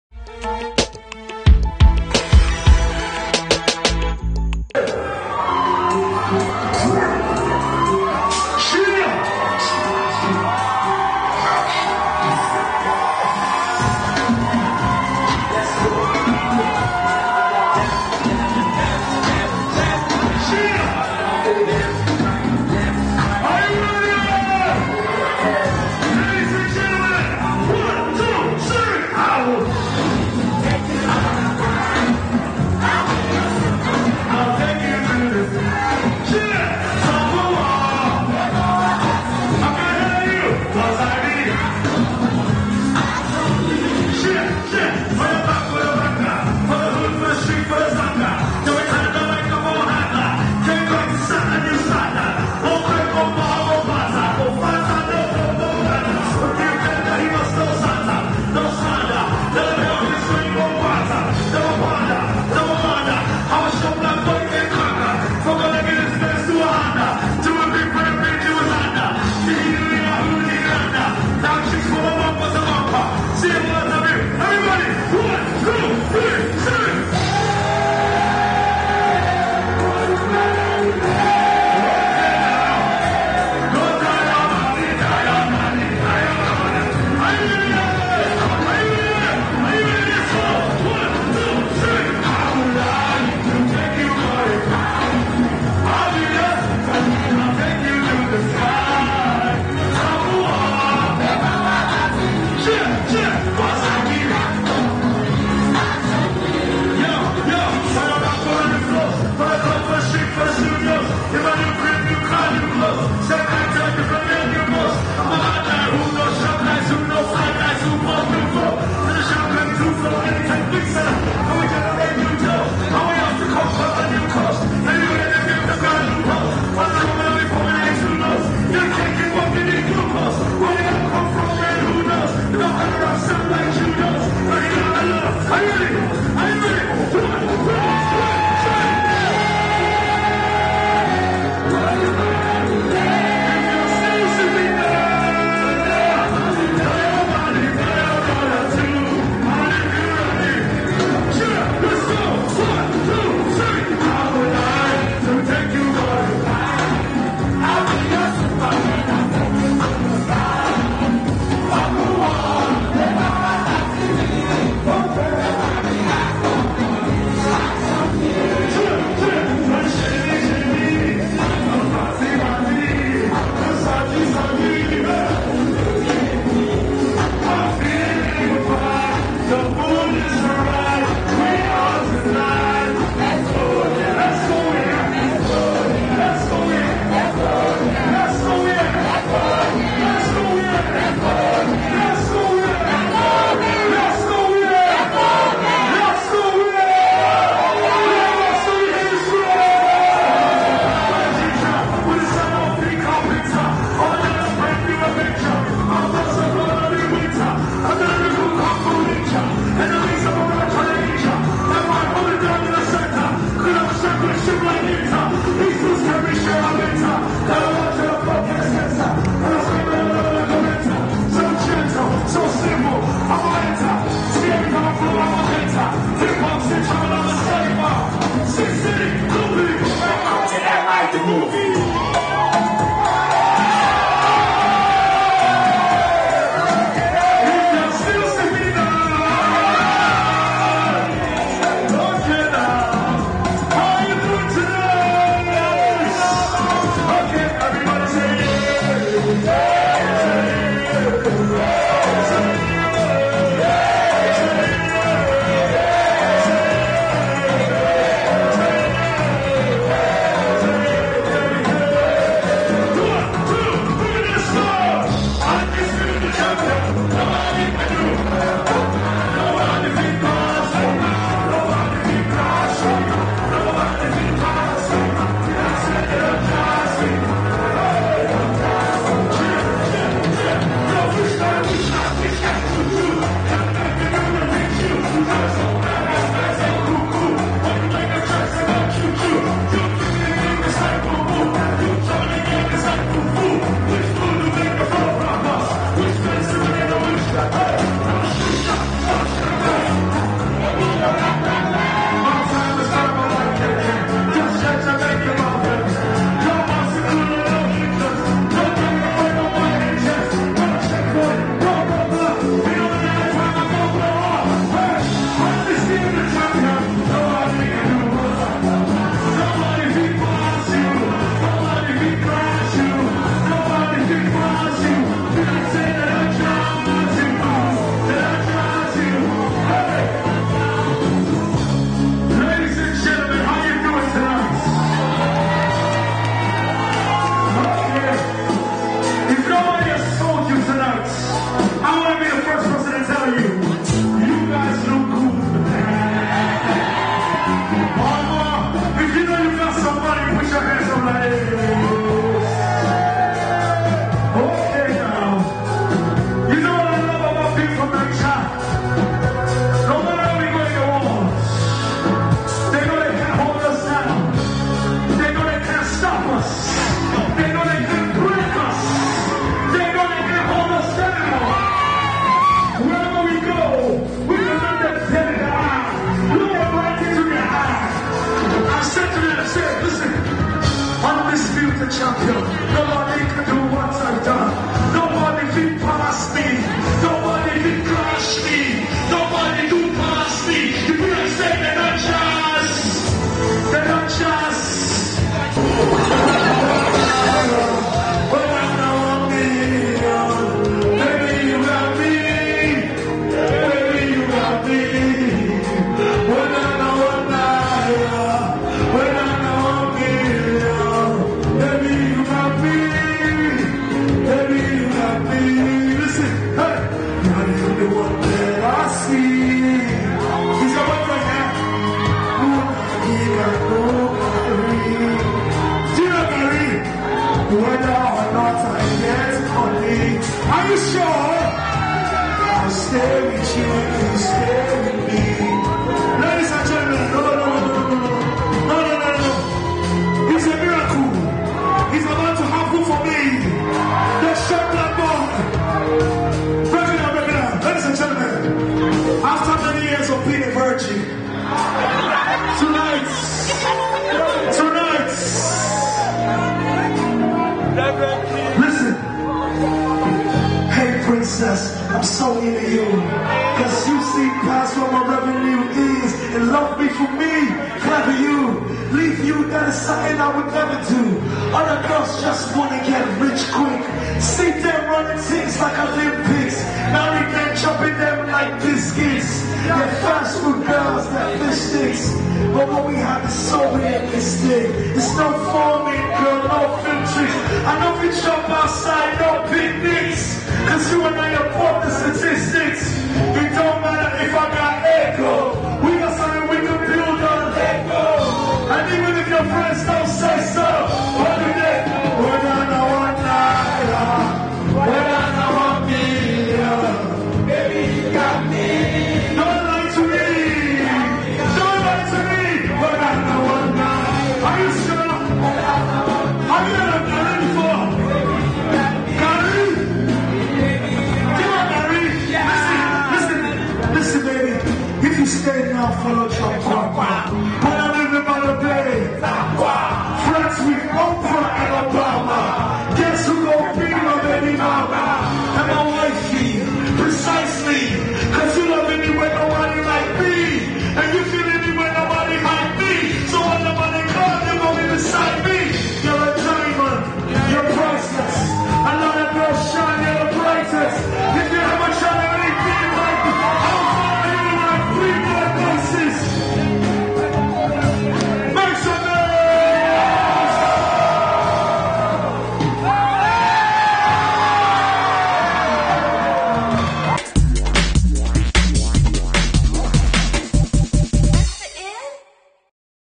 Inspiration FM's Summer Jam - MI
92.3 Inspiration FM, Lagos Nigeria hosted their listeners and clients to Great Music and Comedy this Summer. Channel54 delivers the event to you anywhere, everywhere.